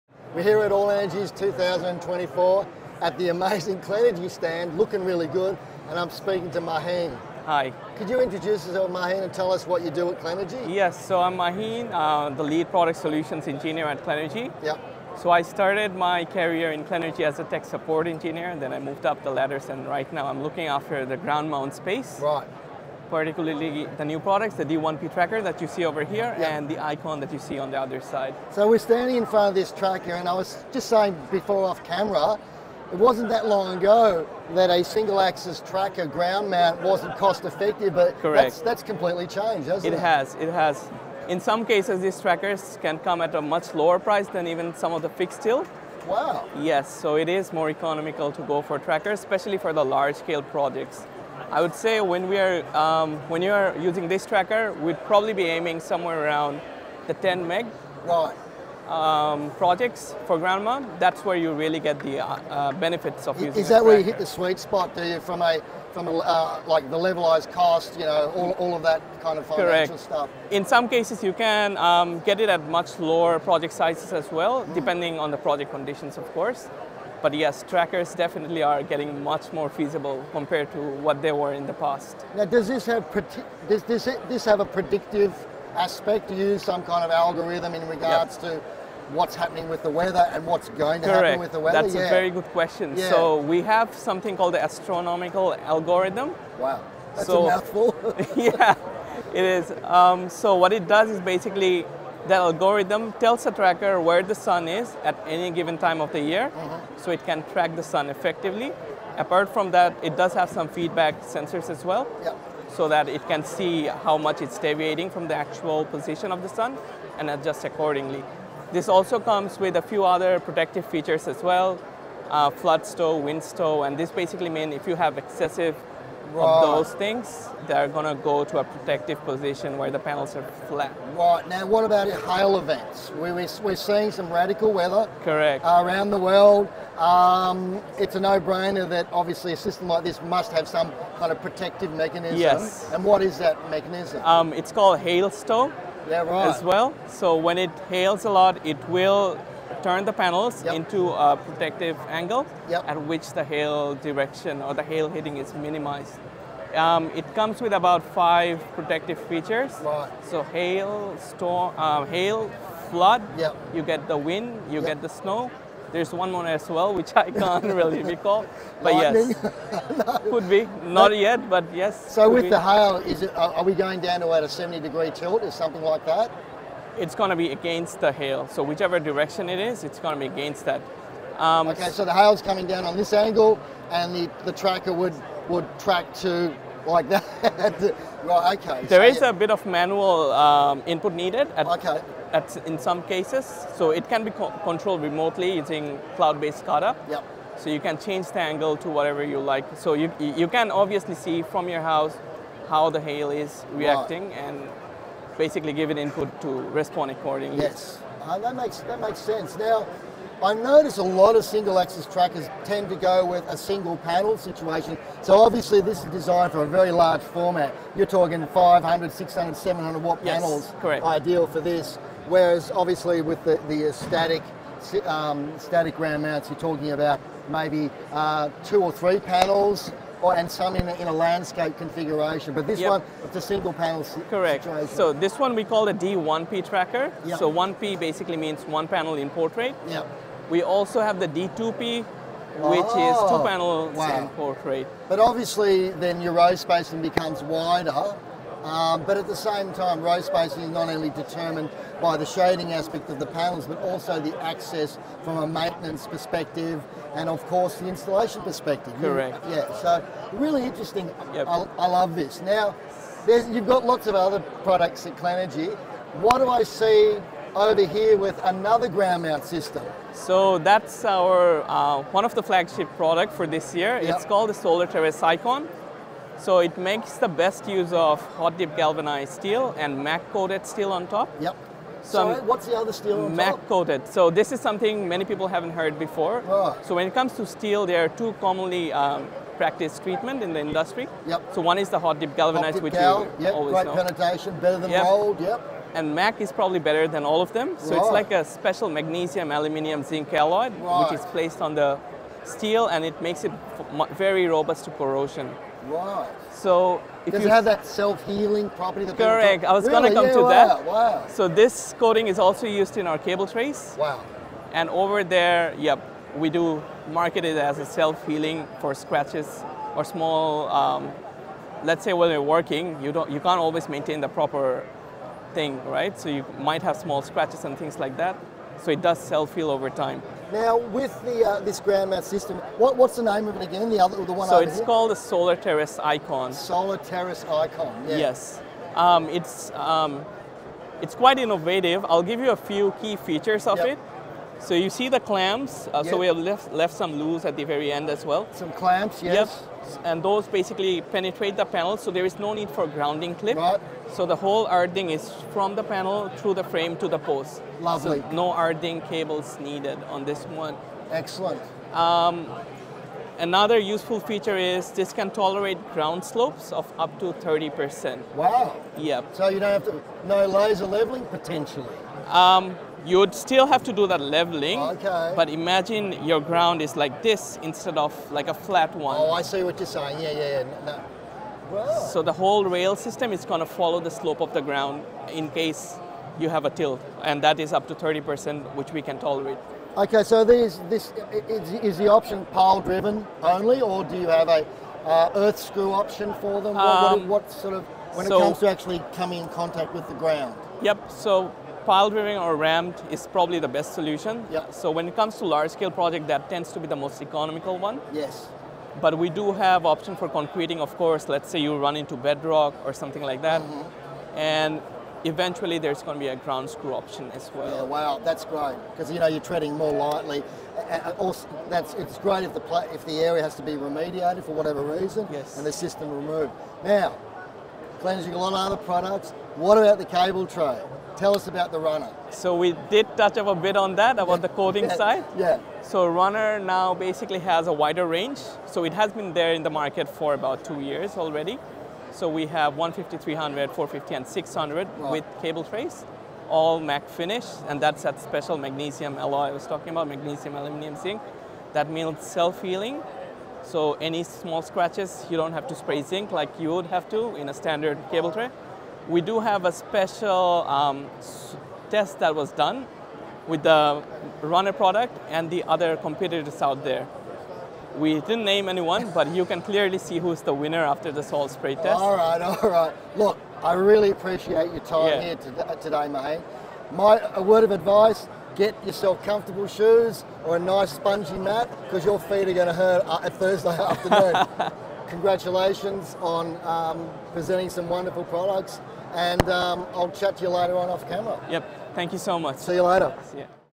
Interview with Clenergy at All Energy 2024
We had a great opportunity to talk to Maheenul from Clenergy at All Energy 2024.